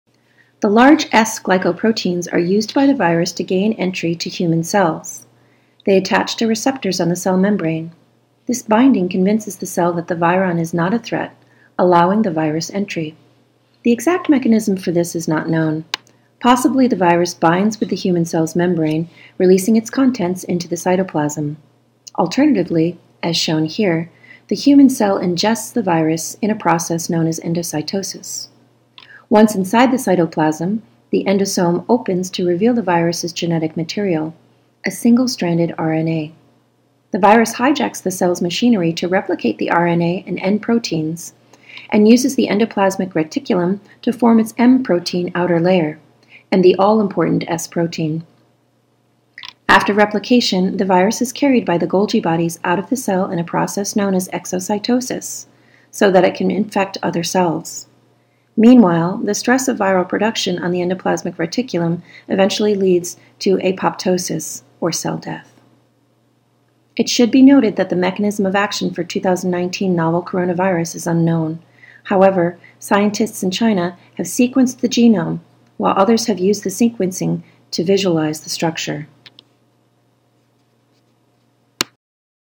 Medical Animation explaining Coronavirus Mechanism of Action - How Coronavirus attacks a human body
While the exact mechanism of action for the 2019 Novel Coronavirus is still unknown, the 3D structure has been published;  This structure shows that the virus is very similar to other coronaviruses and so we show the mechanism of action of how coronavirus gains cell entry, coopts the human cell into producing copies of itself, and causes apoptosis or cell death.

For more info on the biology, the spread and the response to 2019 nCoV see our microsite:

The 3D ribbon structure was taken from Innophore who have created the structure based on the genome sequencing data published by Chinese researchers.  You can find it here: